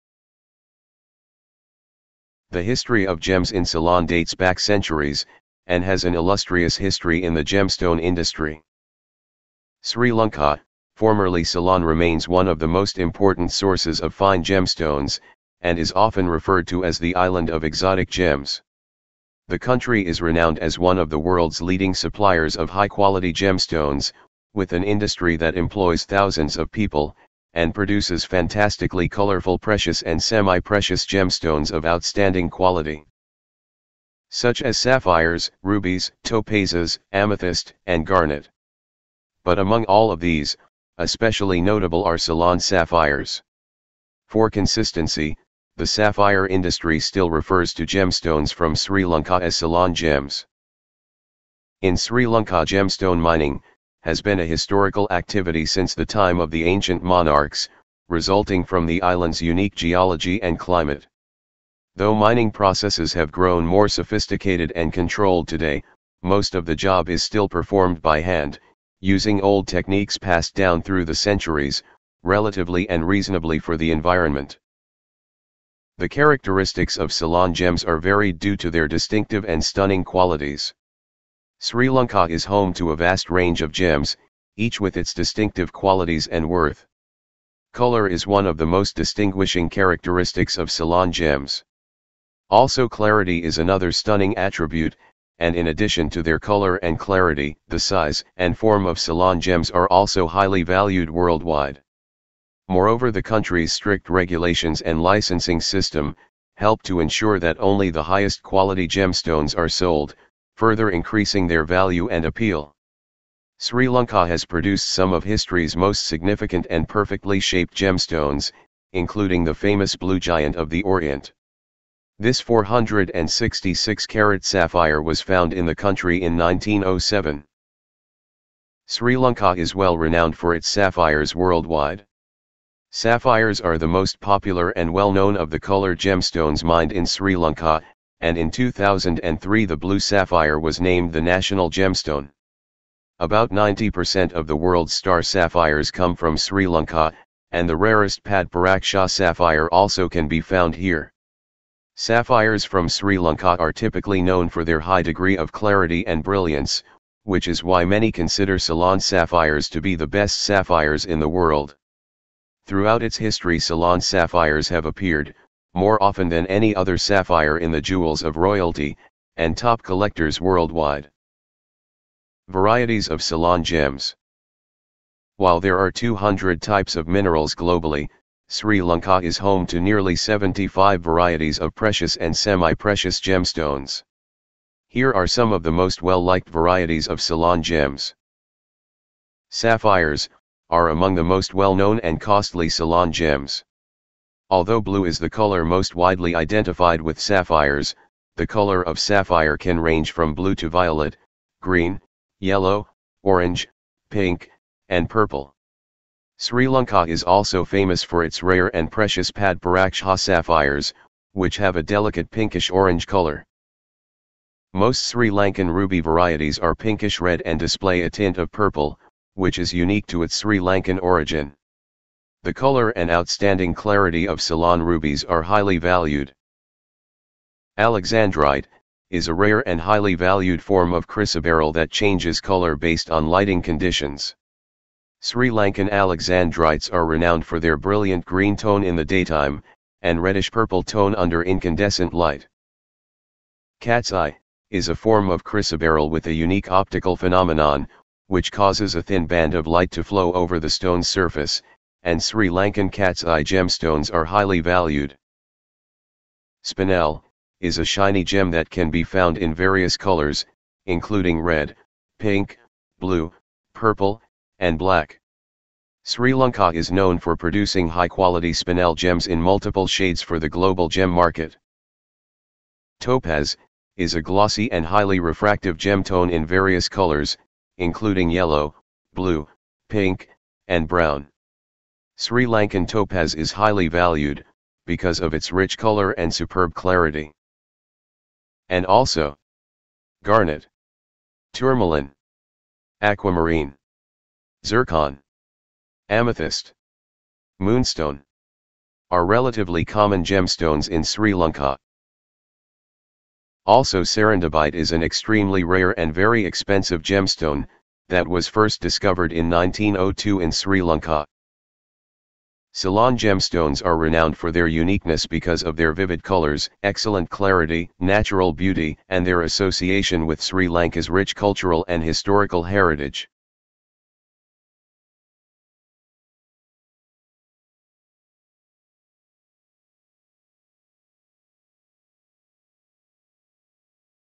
The Gem Island | The History of Gems In Ceylon
The history of gems in Ceylon (Sri Lanka) dates back centuries and has an illustrious history in the gemstone industry.

00:00 - Introduction
01:15 - The characteristics of Ceylon gems
02:08 - Why is Sri Lanka famous for sapphires worldwide?
02:52 - Most well-liked varieties of Ceylon gems
05:12 - What makes Ceylon gems unique?